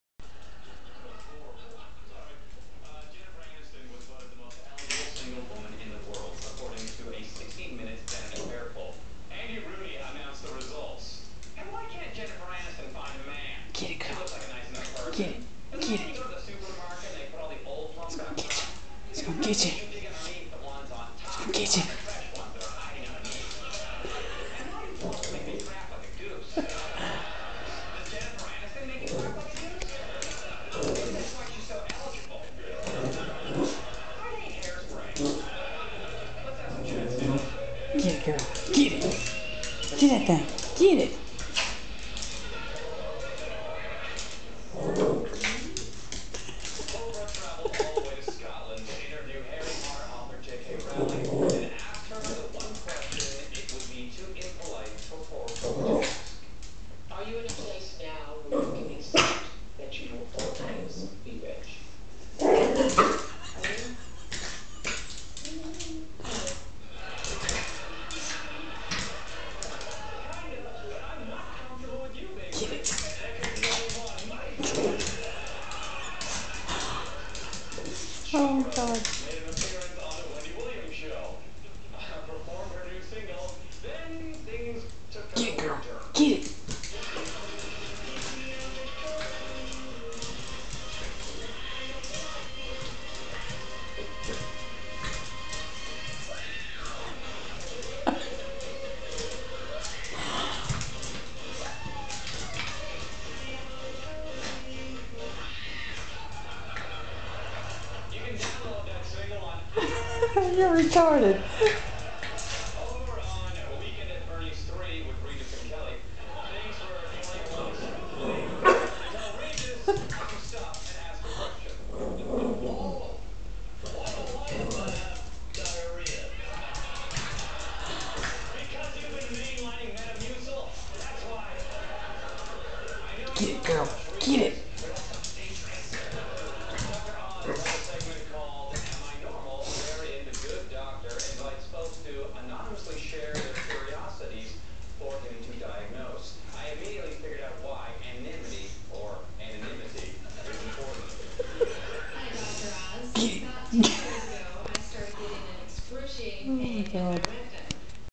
Lucky the Yorkie gets Jiggy with Paper Towel
I have to say our Yorkshire Terrior 'Lucky' has got to be the most entertaining pet we've ever had. She has more moves than Elvis in '56. Check out her getting jiggy with a this balled up paper towel. On into the video she shows off a new move where she backs up to it and does this backwards kick. Cheryl told me she got some good video of my baby girl...and she sure did!